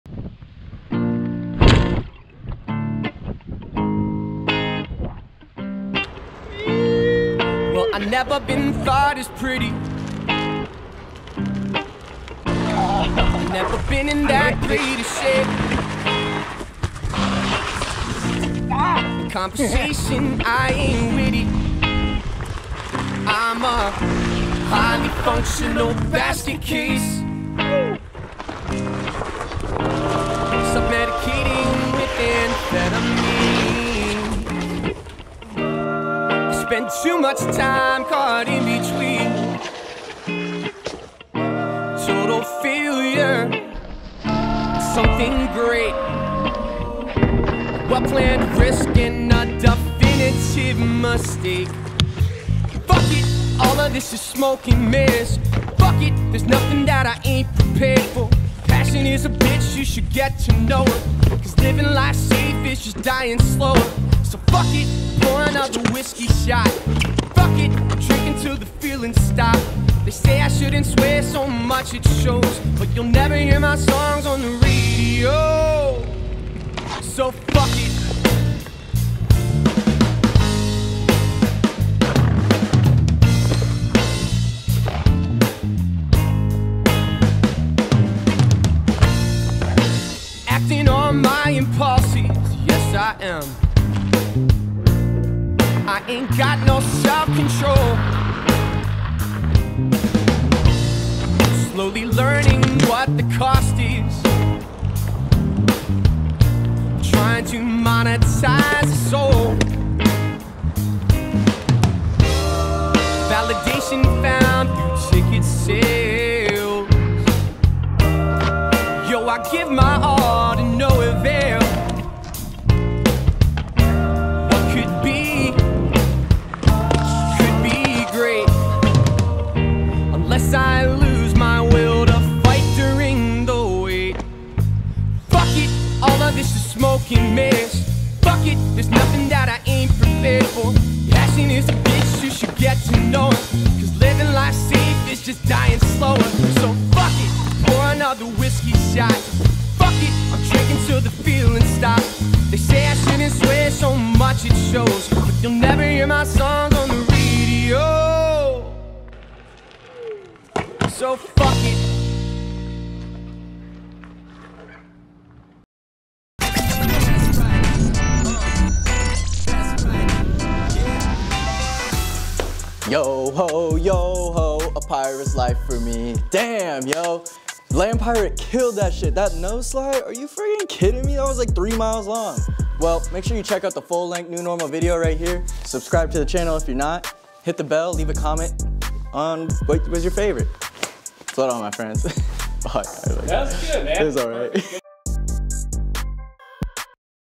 Kyle Taylor's A New Normal Part | Onewheel Tricks
Kyle Taylor aka West Coast Land Pirate's part from "A New Normal"

To learn more about Onewheel Accessories, make sure to visit our Onewheel Accessory Guide